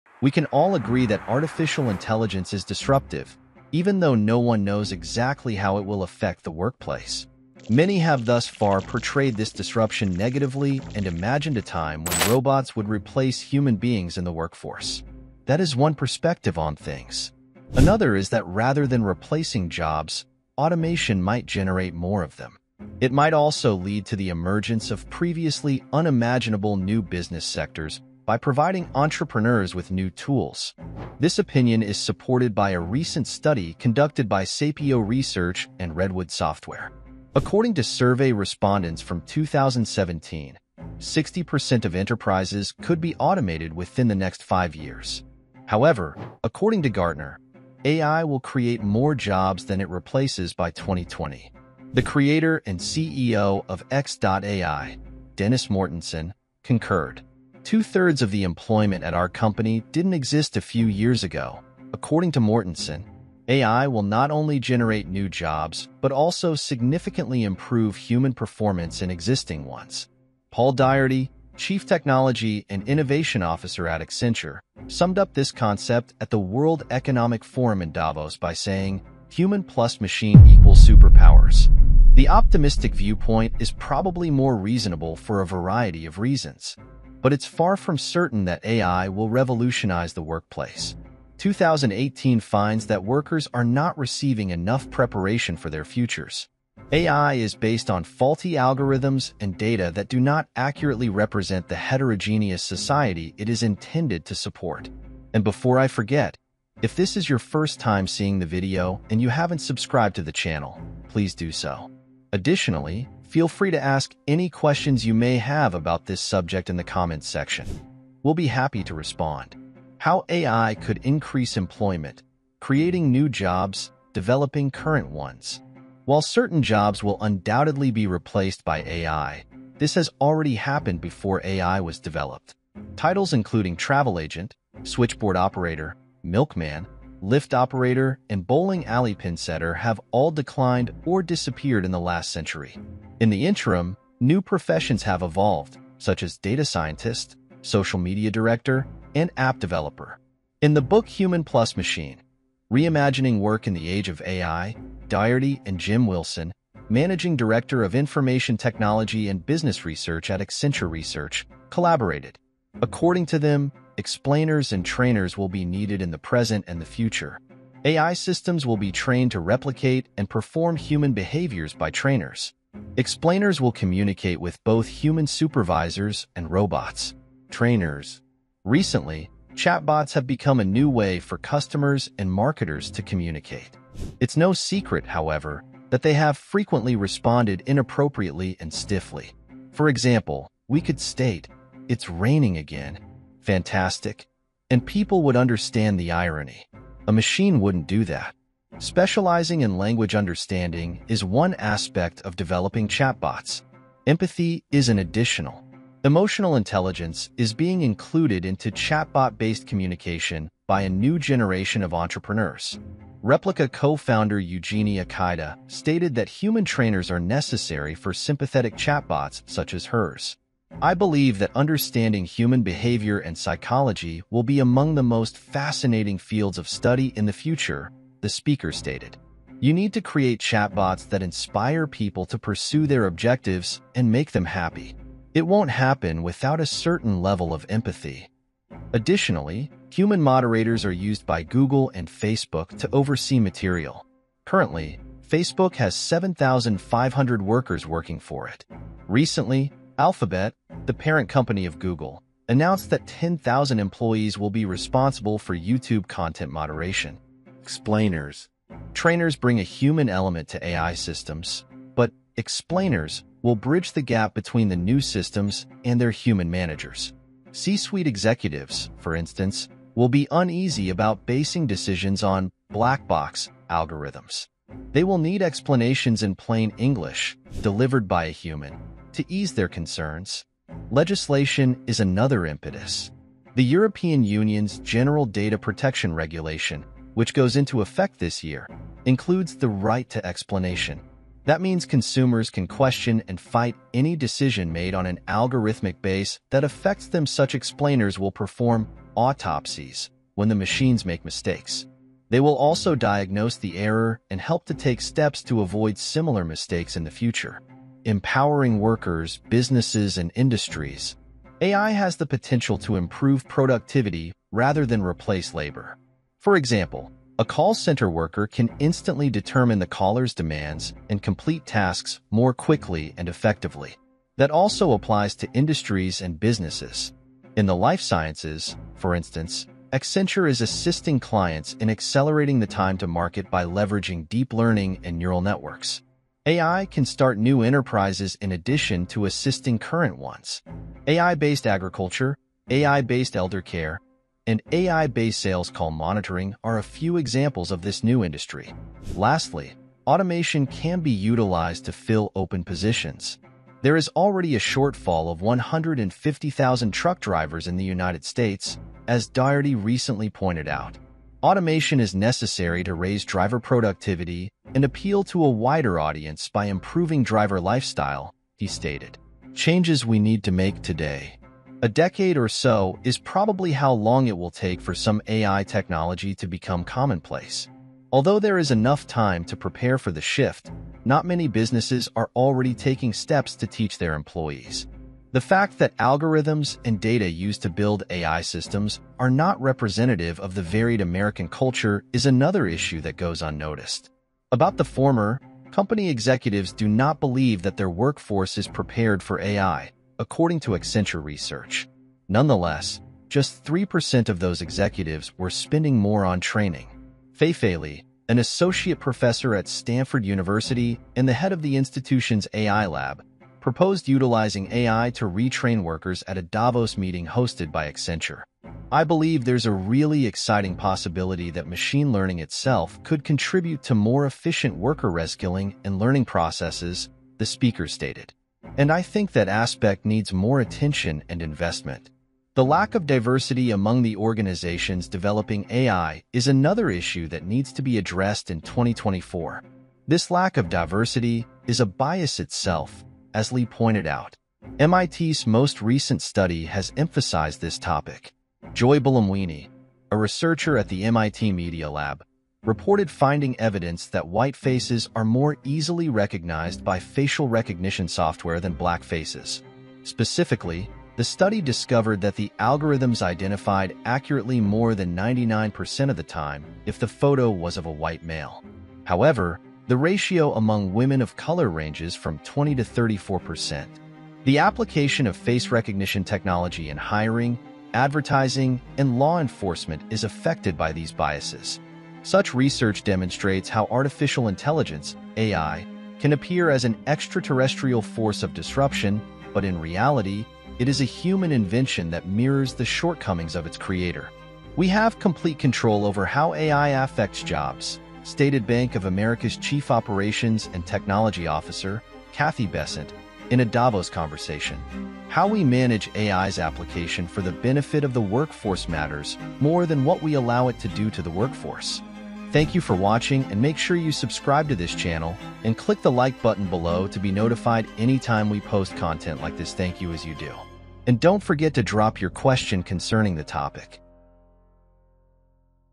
What you don't know about AI and the future of work: 800 million jobs GONE by 2030
AI is changing the future of work, with 800 million jobs predicted to disappear by 2030. Find out what you need to know about AI and its impact on employment in this eye-opening video.